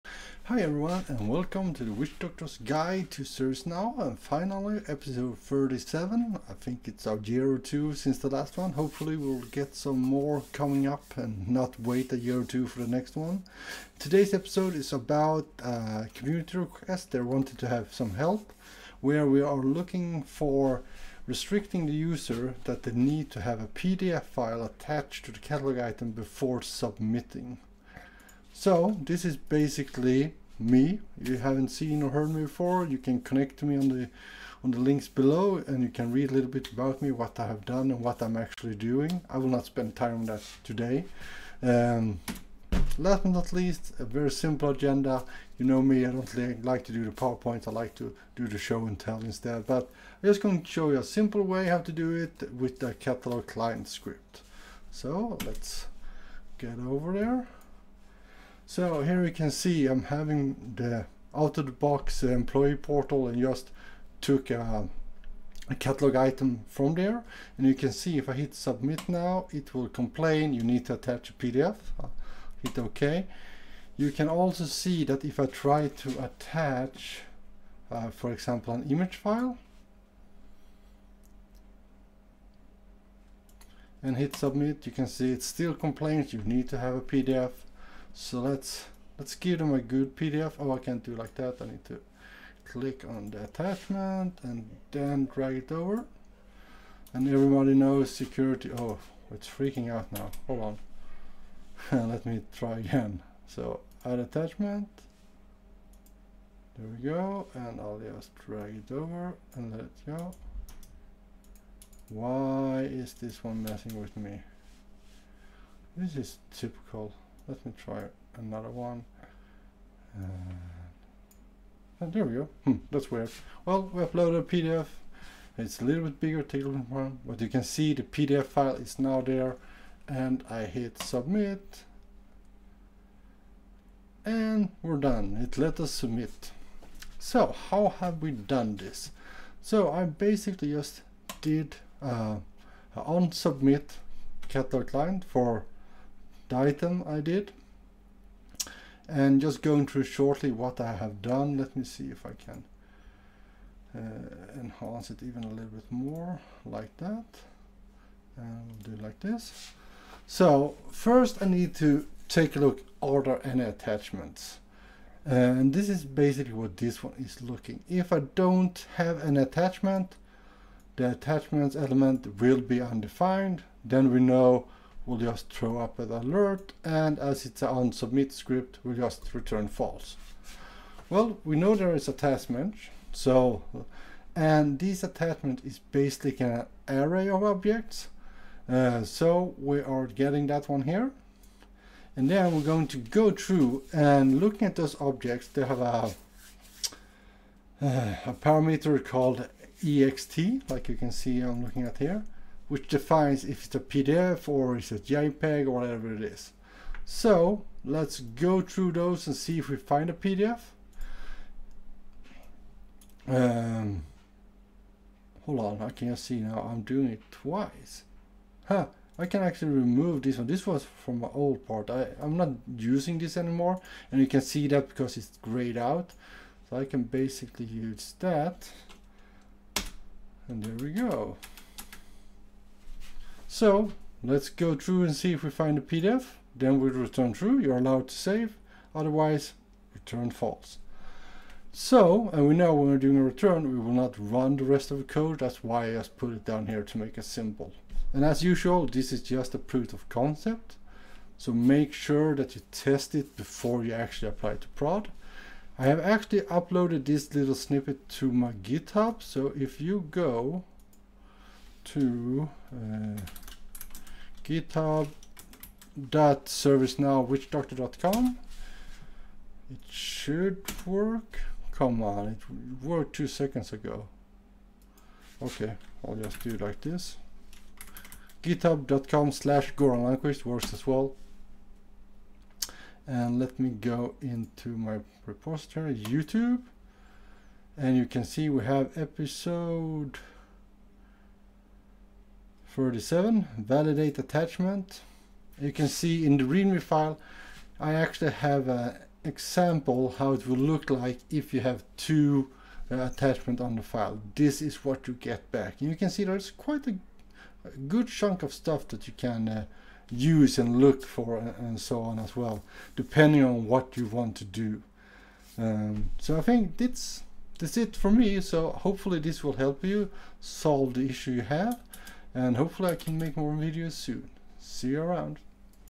Episode 37 - Require PDF attachment to submit Catalog item in Vancouver release
This Catalog Client Script enables users to submit a catalog item only after attaching a PDF file, as per community request.

Video recorded in Vancouver release